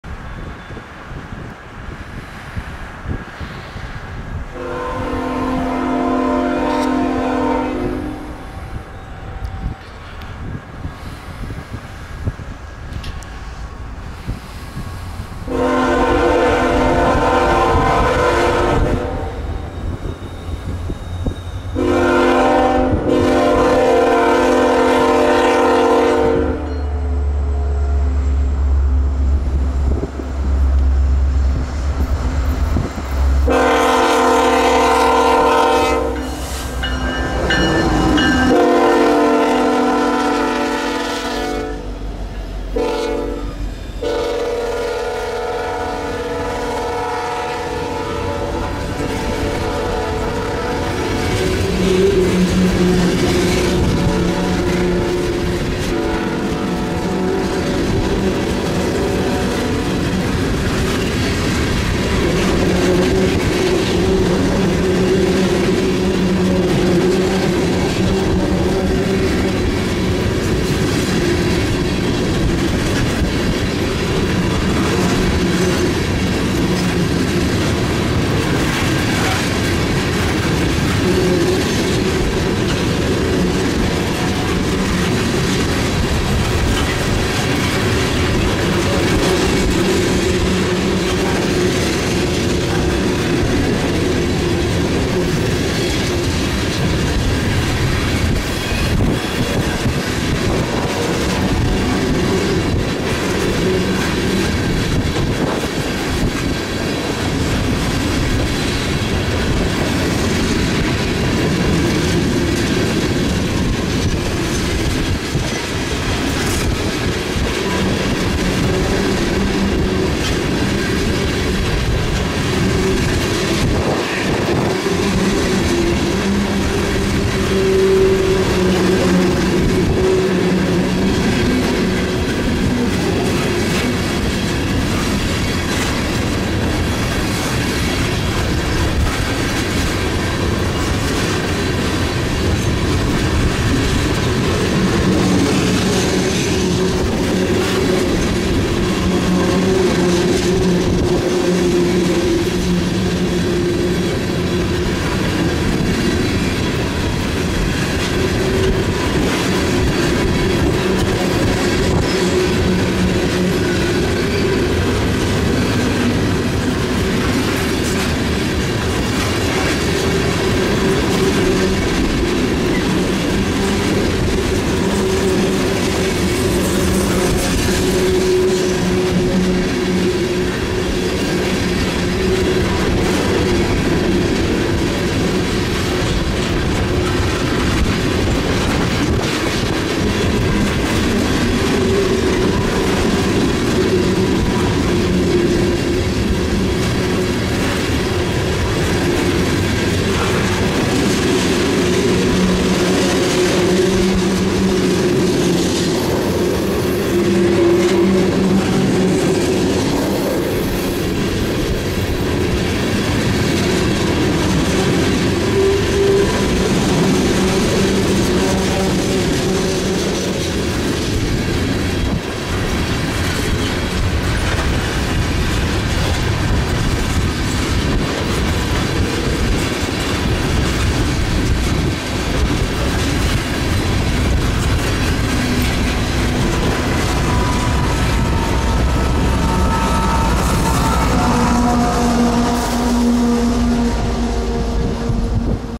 BNSF 3892 w/ CP Power- Ethanol Train, Davenport, IA
1/5/22- BNSF 3892 (ET44C4) & 8847 (SD70MAC) lead an ethanol train south. CP 8780 is the DPU.